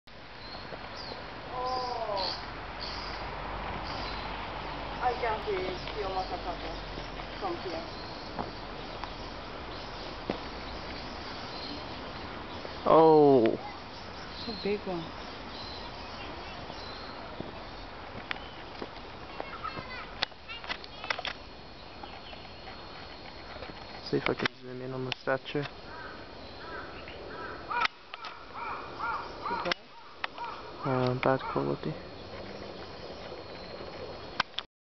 Kiyomasa Kato Statue - Pt.1
Part one
walking up the 300 steps leading to the statue of Kiyomasa Kato
behind Honmyoji Temple in kumamoto city Kyushu Japan